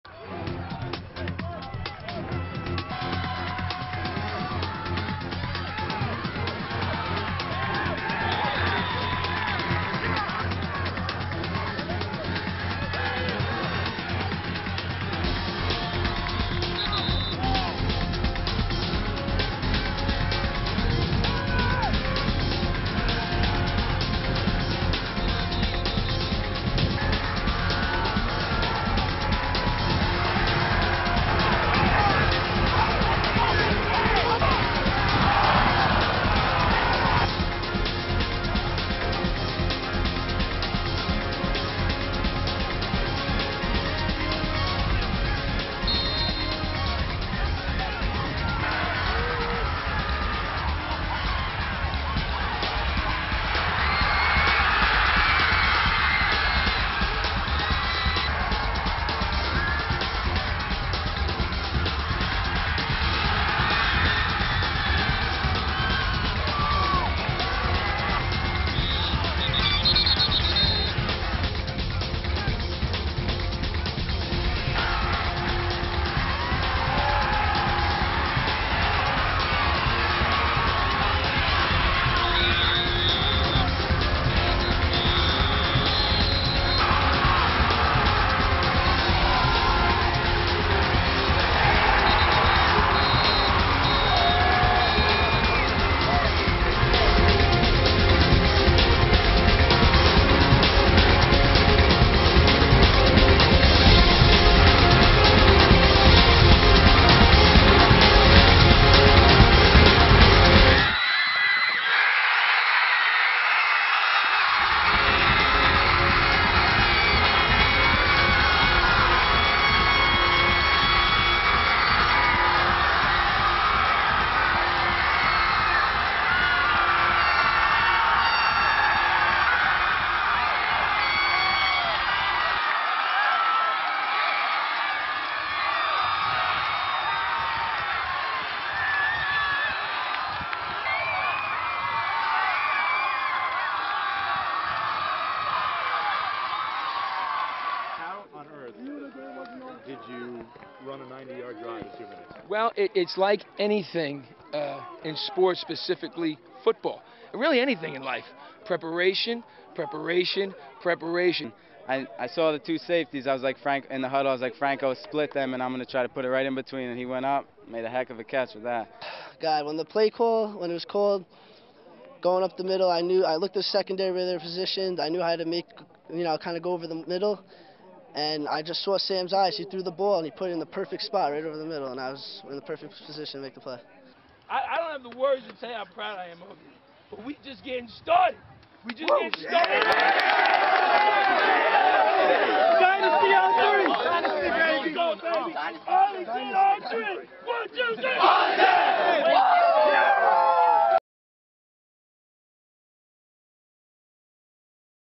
Two Minutes To Glory
Arlington Football down 20-10 in the 4th quarter - marches 90 yards and makes a miraculous comeback to beat John Jay on the final play of the game! check out